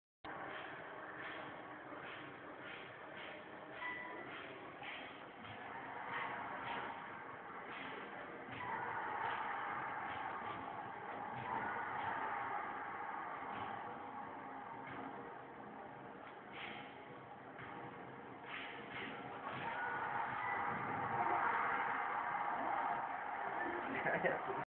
ell ferrr boxeandoo
fer en mi ksaa boxeando wii mientras yo staba enfermooo asi m kuidaba ojjoj